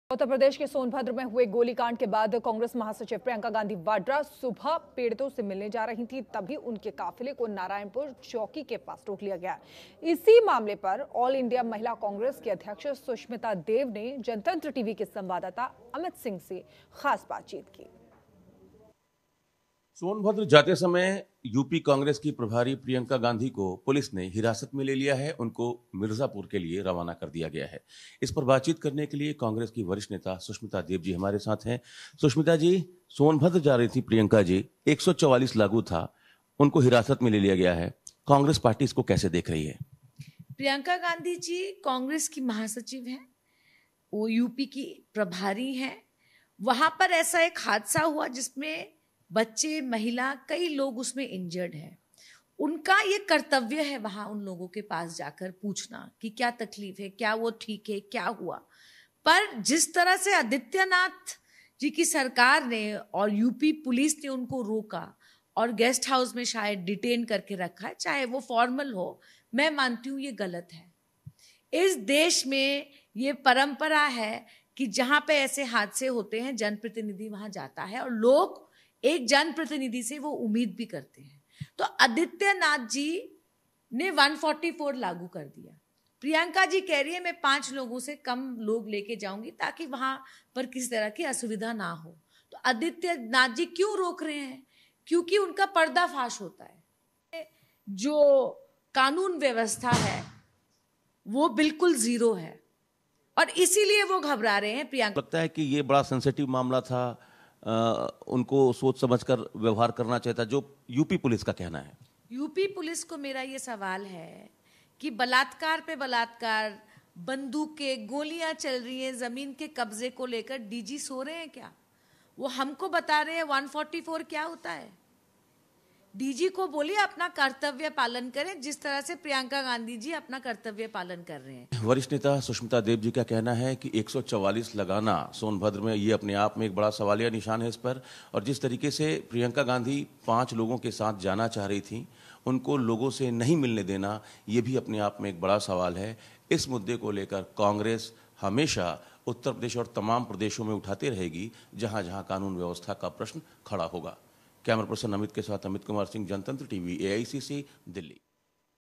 Sushmita Dev ने की Jantantratv से खास बातचीत
महिला कांग्रेस अध्यक्ष सुष्मिता देव ने जनतंत्र टीवी से खास बातचीत की। इस दौरान उन्होंने क्या कुछ कहा, देखिए इस वीडियो में...